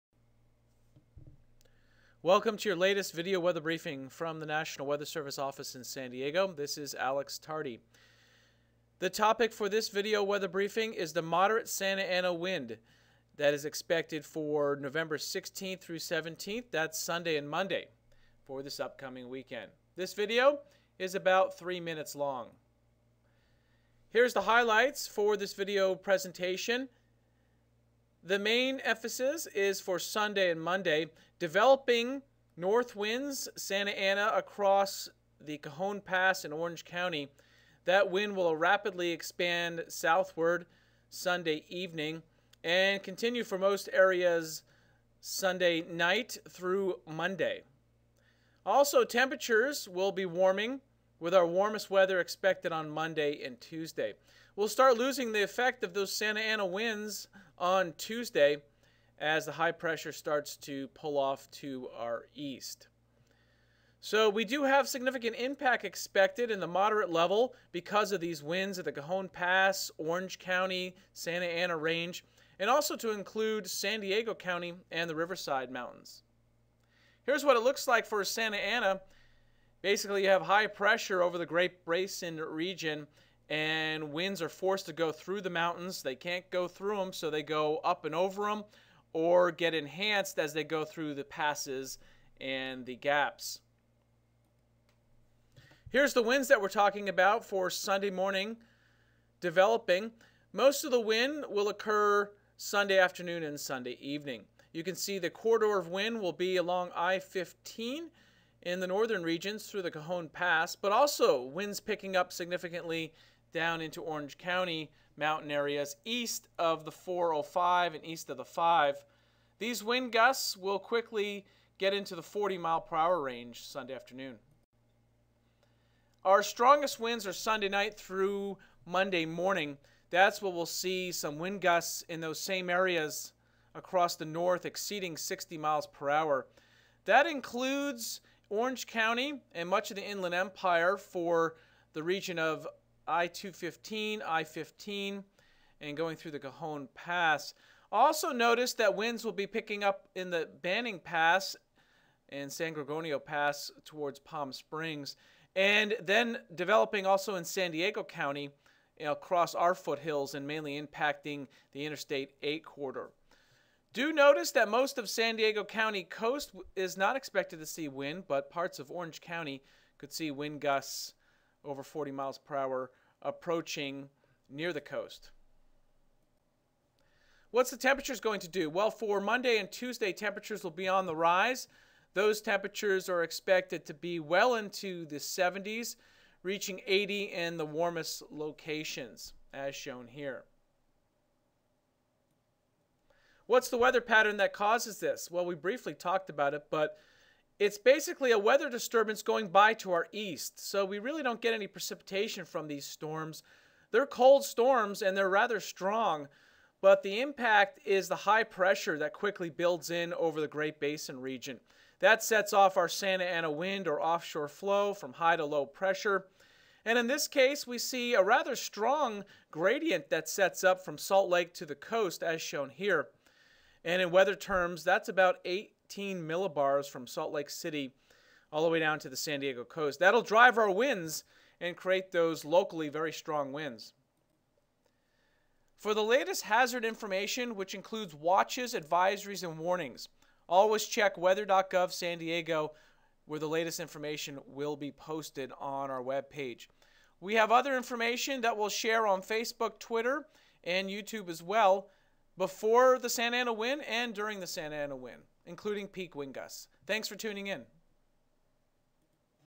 Moderate Santa Ana Wind for Southern California November 16-17 - NWS San Diego
Offshore, Santa Ana, wind will develop on Sunday and continue on Monday. This will bring breezy to locally windy conditions to many areas except the San Diego coastal regions. It will also result in very low humidity, or dry air, and warm temperatures. The wind will decrease by Tuesday and be followed by a slow cooling trend.